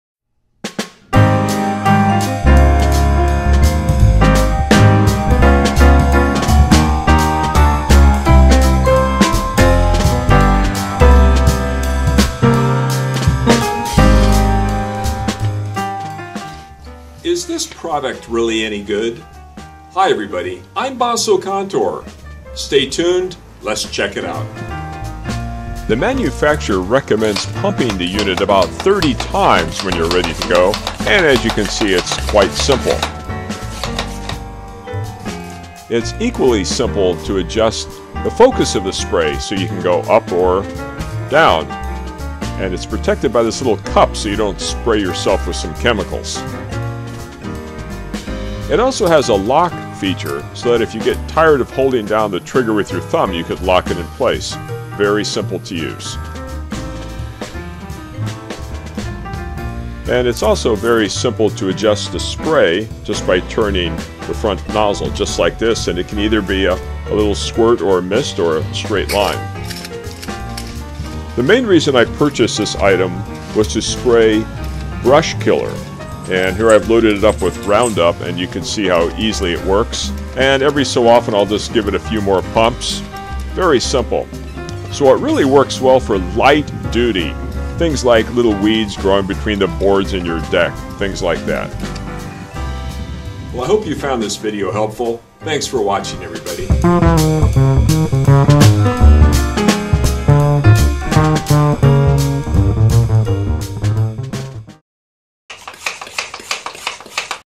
Solo 418 Garden Pump Sprayer, 1 Liter
Solo Model 418 Garden Pump Pressure Sprayer, 1 Liter.

Note: This is NOT an advertisement. I paid full price for this item.

This model is good for small jobs around the yard.  You simply add the pre-mixed solution, pump the unit about 30 times (easy), and you're ready to go. I purchased this item to spray weed killer. It worked well.

There is a locking tab, so you don't have to constantly hold down the trigger. You can adjust the spray form, as well as the direction of the spray.